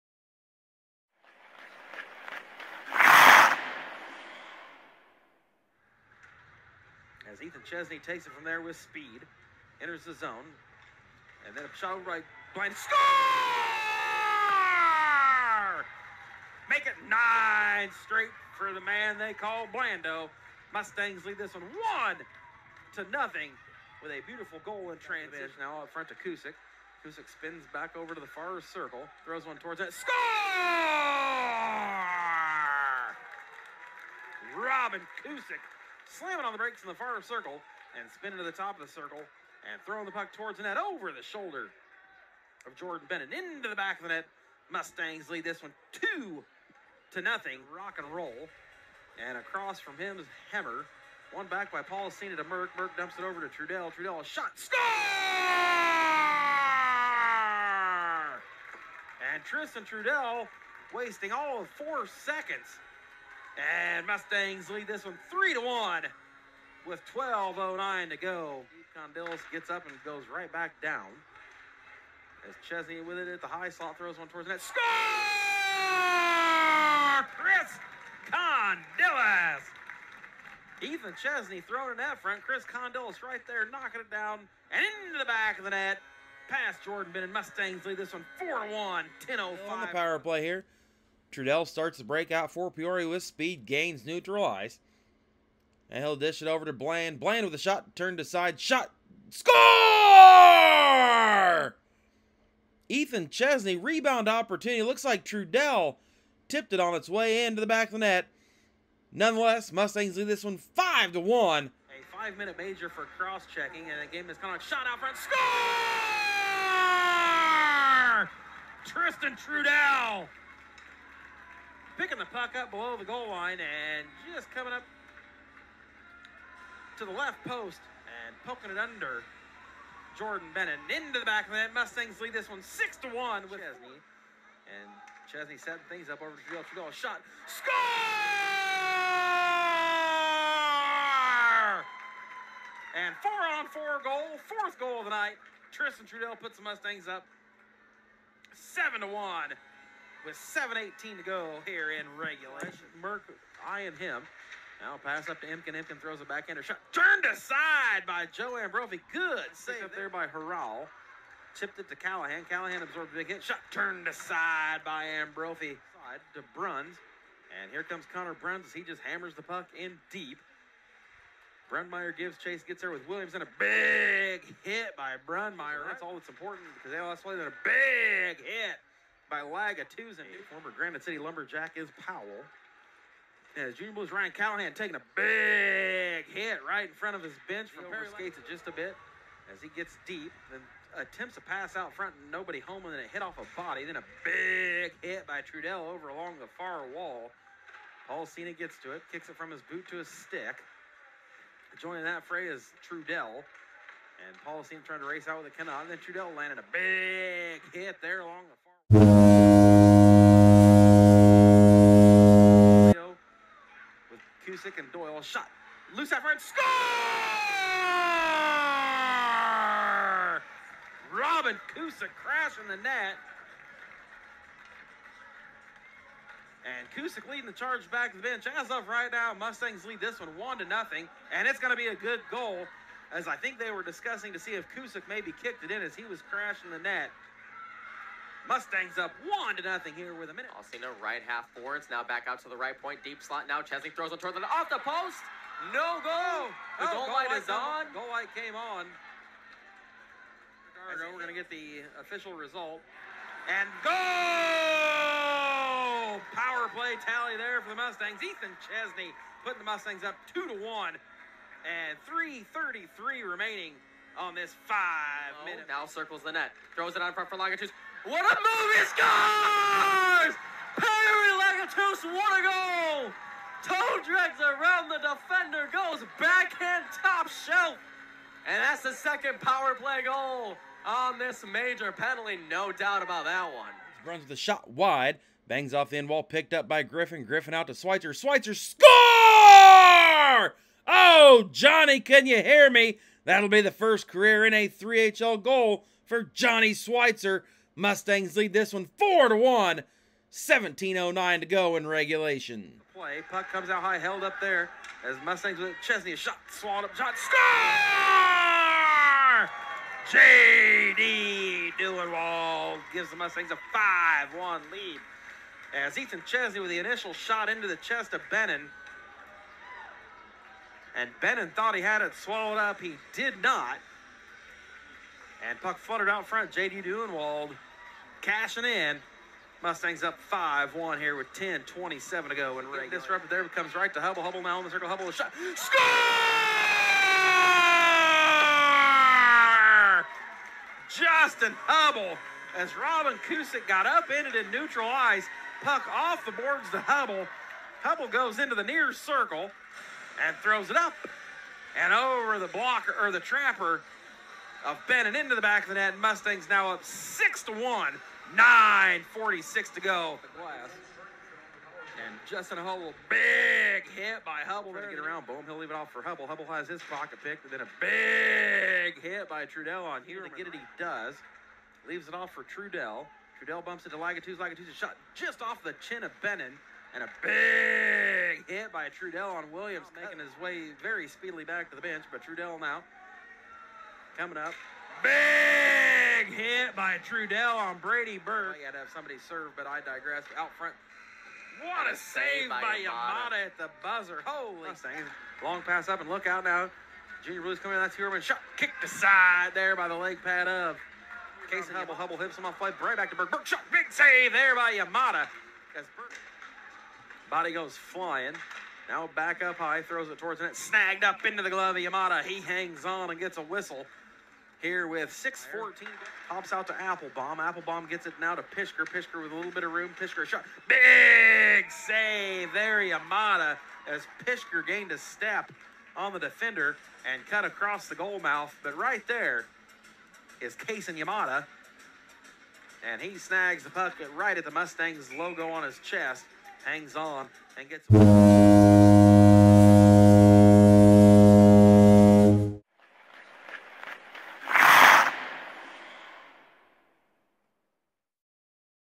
Weekend Highlights from 11/19-20 vs St. Louis Jr Blues
Relive the Mustangs 7-1 and 6-1 victories over their long time rival St. Louis Jr Blues at the Owens Center on 11/19/22 and 11/20/22.